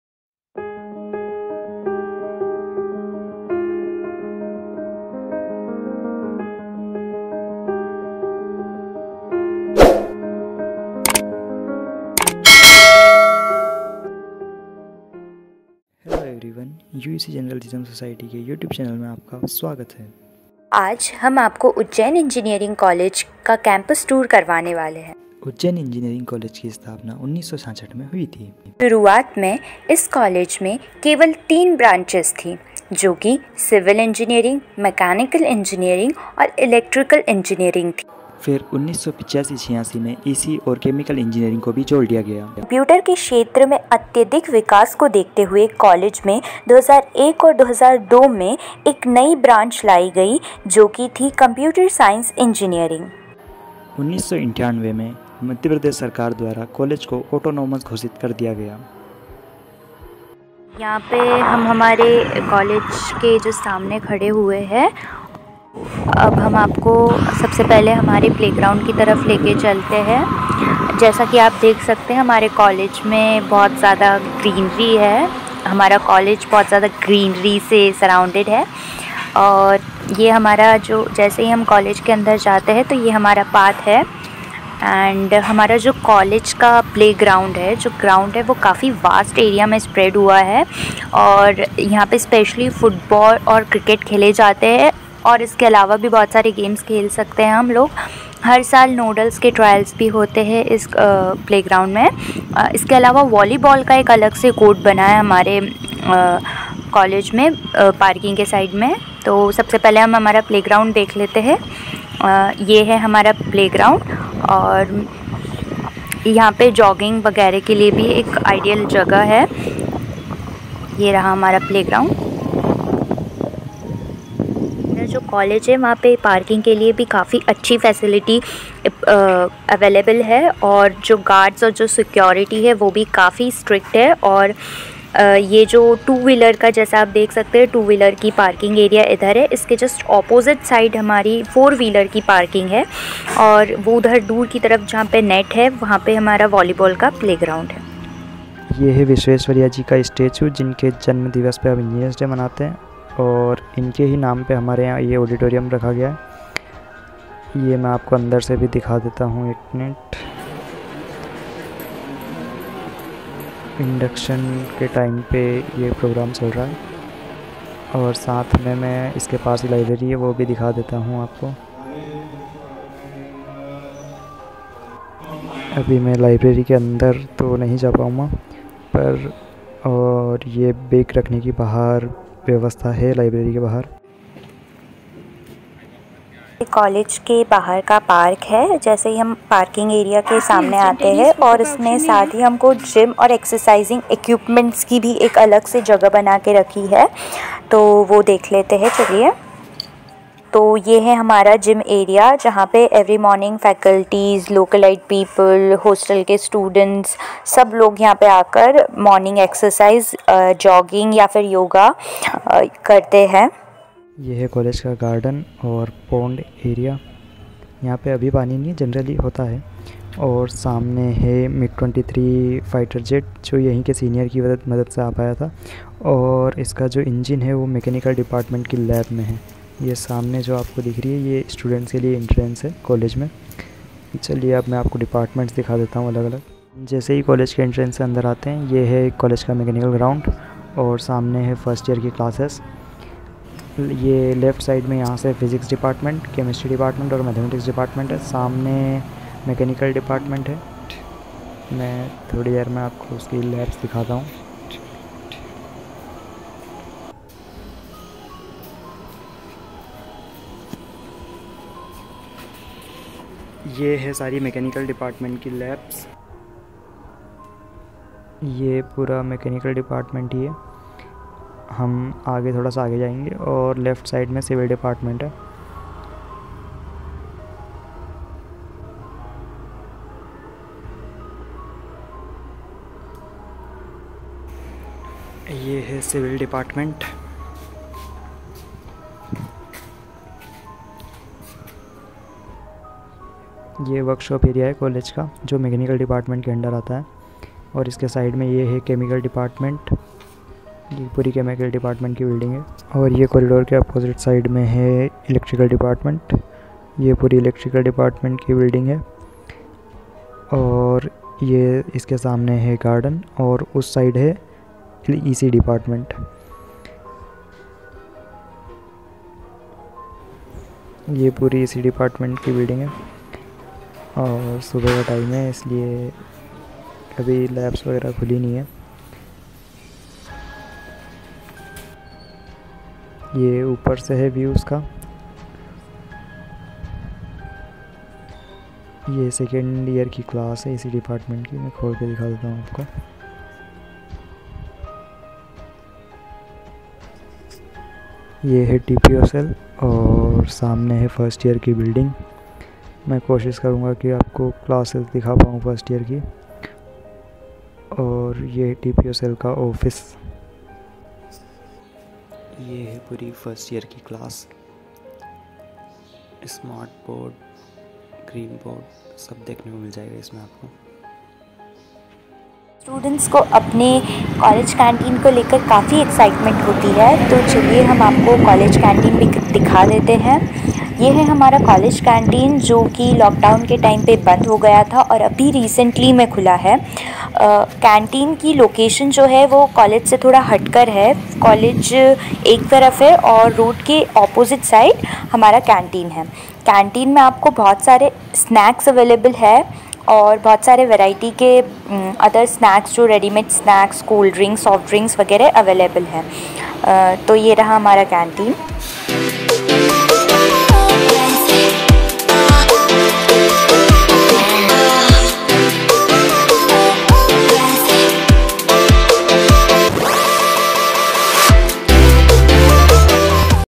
CAMPUS tour of Ujjain Engineering College | Uecu | Gecu | Government Engineering College
Campus tour by Minakshi & Yuvraj .